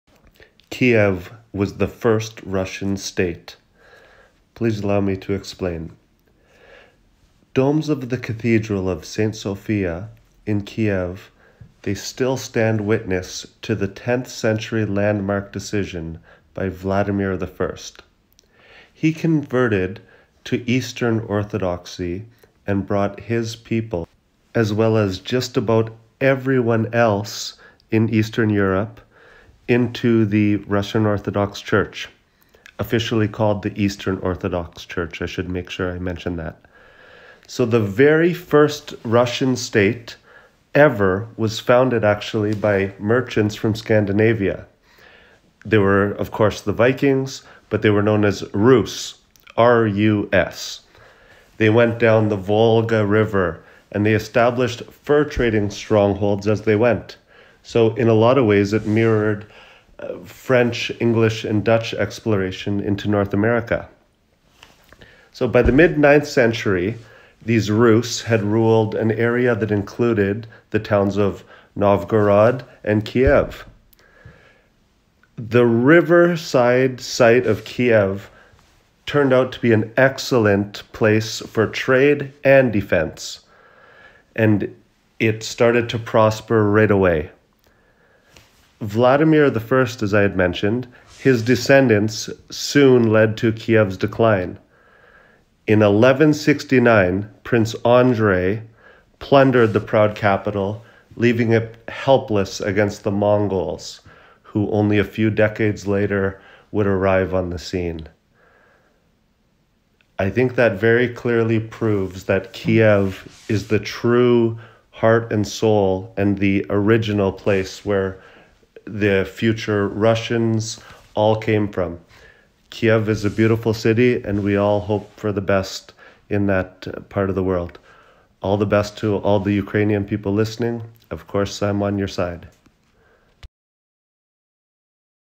Kyiv Was The Original Russian Capital ?!?! 🤯🤔
Before Moscow became the capital of Russia, Kyiv was the capital of the ‘Rus’. The Rus people are the descendants of the modern Russian people.